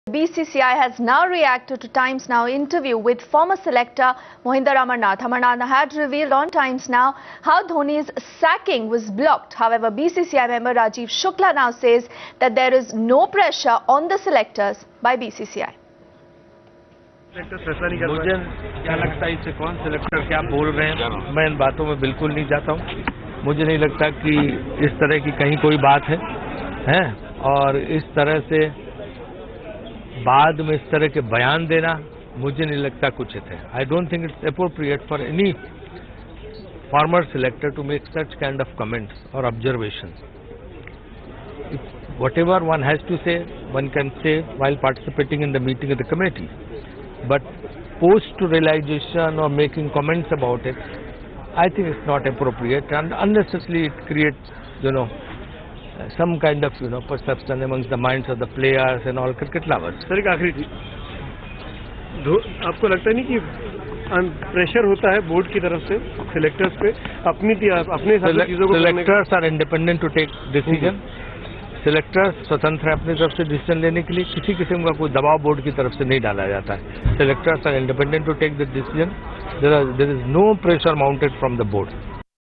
BCCI refutes Mohinder Amarnath
Senior BCCI official Rajiv Shukla BCCI official Rajeev Shukla reacted on former national selector Mohinder Amarnath's TIMES NOW's interview where he admitted how Mahendra Singh Dhoni's sacking was stopped despite three selectors agreeing for it. Shukla said there is no pressure on the selectors by BCCI officials.

For the first time since he was axed from the selection committee, Amarnath in an explosive interview to TIMES NOW, has revealed how Dhoni's sacking was blocked despite three selectors agreeing to wield the axe Download the Times Now India's Election HQ app and get all the election info at one go. Social Media Links